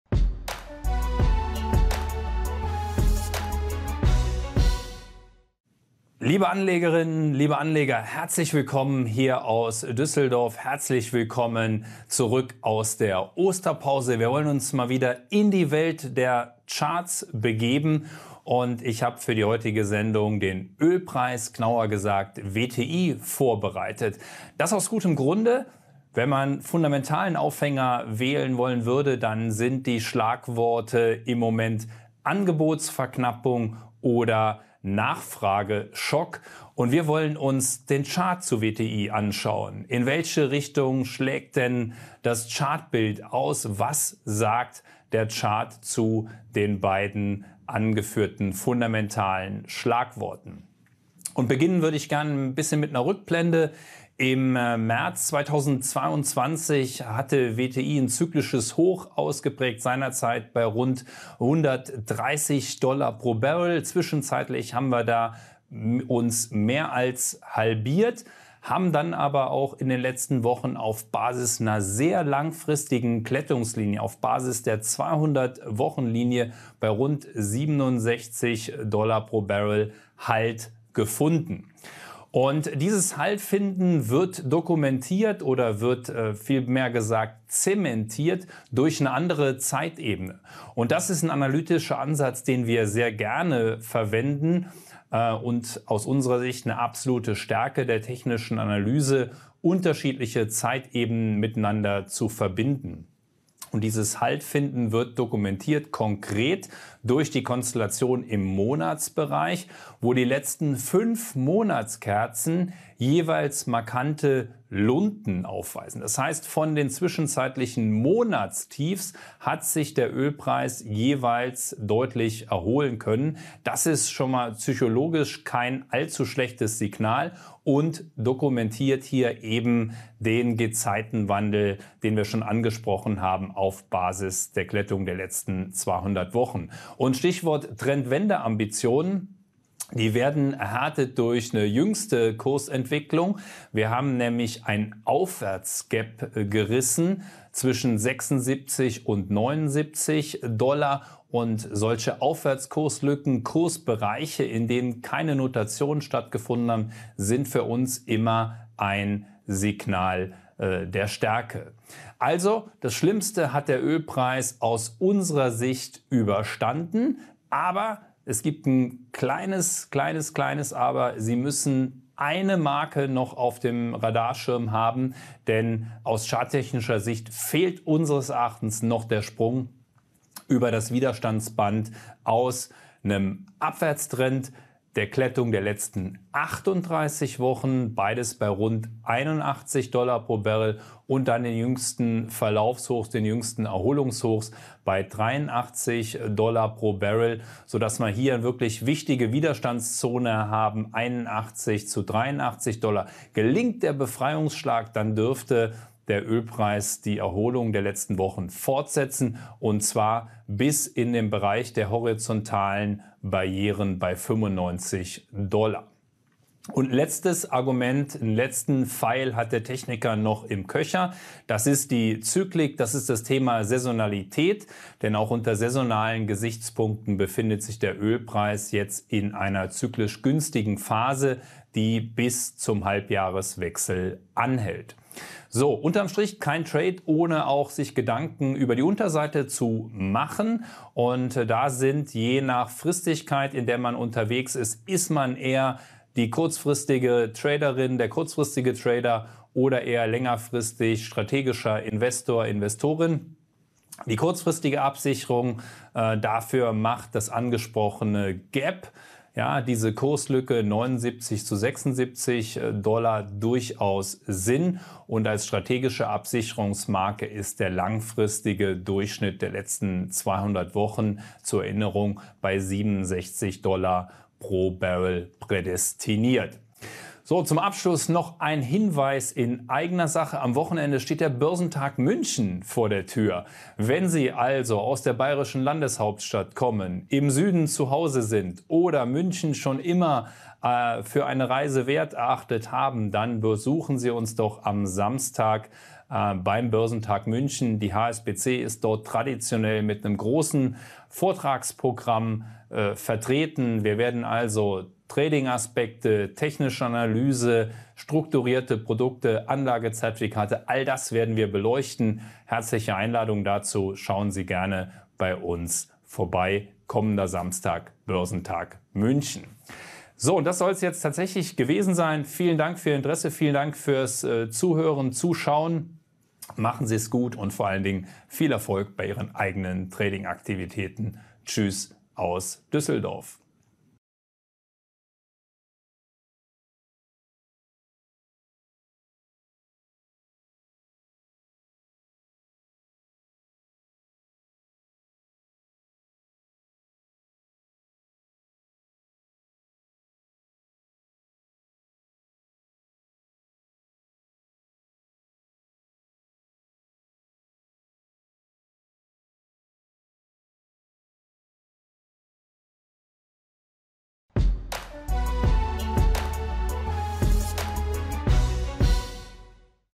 Ölpreis im Chart-Check: Schlüsselmarke voraus! - HSBC Daily Trading TV 18.04.23
In dieser Ausgabe von HSBC Daily Trading TV analysiert Jörg Scherer, Leiter Technische Analyse, den Ölpreis der Sorte WTI.